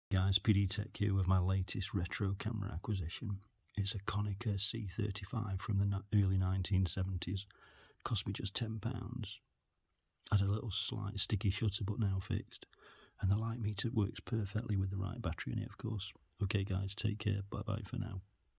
My Latest Retro Vintage £10 Camera - Konica C35 Automatic
My latest bargain retro camera find, a £10 Konica C35 Automatic Vintage rangefinder camera from the early 1970s.

It's in stunning condition but had a sticky shutter through lack of use now sorted and the built in light meter seemed to be reading constantly high.
Most likely caused by the incorrect LR44 1.5v battery fitted, it should have a 1.3v Mercury cell which they don't make anymore so the closest I found was a 1.4v hearing aid 675 battery, hopefully that will give a better reading?